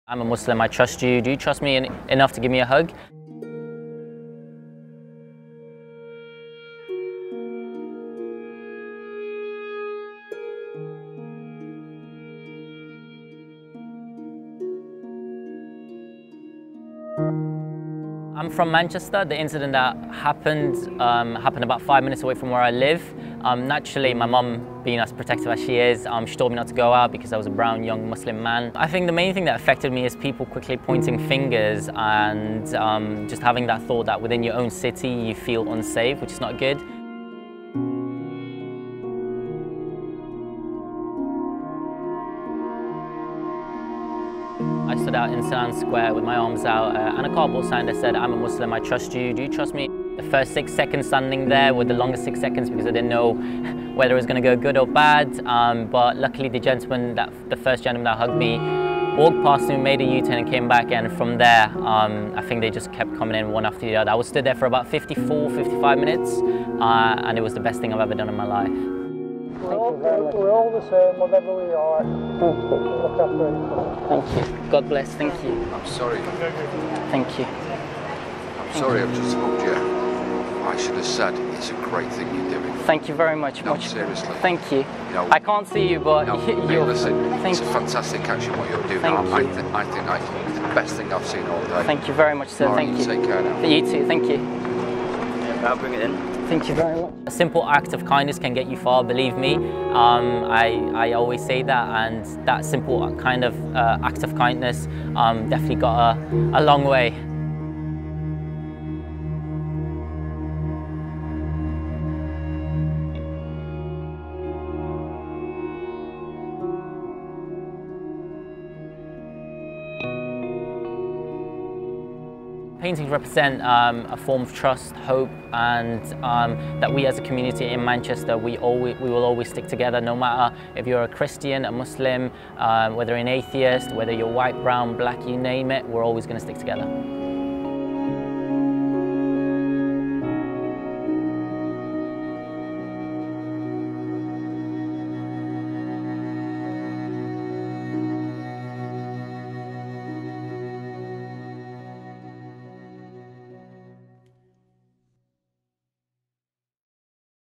Muslim man who offered hugs after Manchester attack reflects a year on | ITV News
A Muslim man's 'act of kindness' following the Manchester terror attack a year ago has been recognised in a series of art displays around the city.

Three days after a suicide bomber in the Manchester Arena killed 22 people last May, 23-year-old Baktash Noori, who vlogs as Bako, stood blindfolded in the city centre offering people hugs.

He told ITV News: “My mum being as protective as she is, she told me not to go out because I was a brown young Muslim man.

“I think the main thing that affected me was people quickly pointing fingers and having that thought within your own city you feel unsafe, it’s not good.

“I stood out in St Ann’s Square, with my arms out and a cardboard sign that said ‘I’m a Muslim, I trust you. Do you trust me?’”

The city’s response to Bako’s gesture was heartening.

In the video recording posted on YouTube, one Mancunian is heard telling the vlogger: “It’s a fantastic catch in what you are doing.

“And I think it’s the best thing I’ve seen all day.”

Another passerby told him: “We’re all the same wherever we are.”

His interactions have been captured by artist Ghislaine Howard, with her paintings on show in the city.

“The paintings represent a form of trust, hope and that we as a community in Manchester, we will always stick together, no matter whether you’re a Christian, a Muslim, whether you’re an atheist, whether you’re white, brown, black you name it, we’re always going to stick together,” Bako said.

The artwork is part of a series of anniversary events being held across the city as it stands in solidarity with those who lost loved ones in the attack.